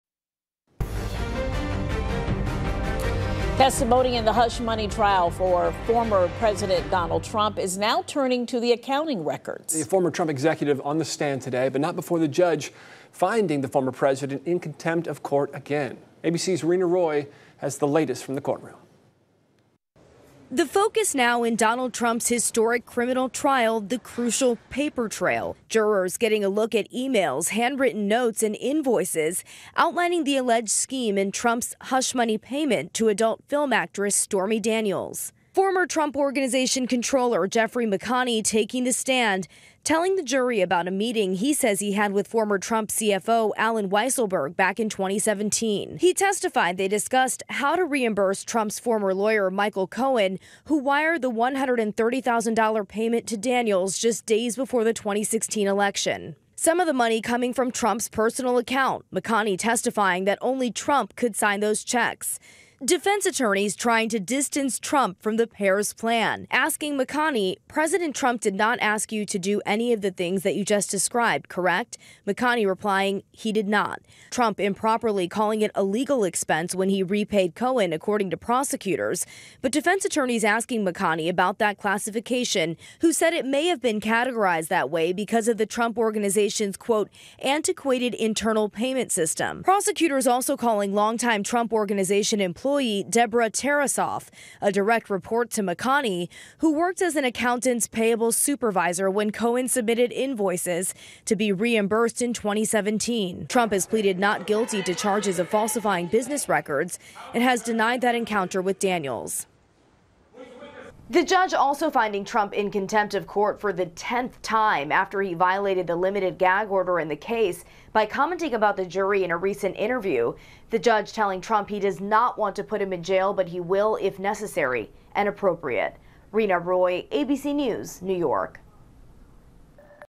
Revealing account records in Trump trial
Testimony in the hush money trial of former President Donald Trump is now turning to the accounting records. A former Trump executive took the stand on Monday, but not before the judge found the former president in contempt of court, again. ABC's Reena Roy has more from the courtroom.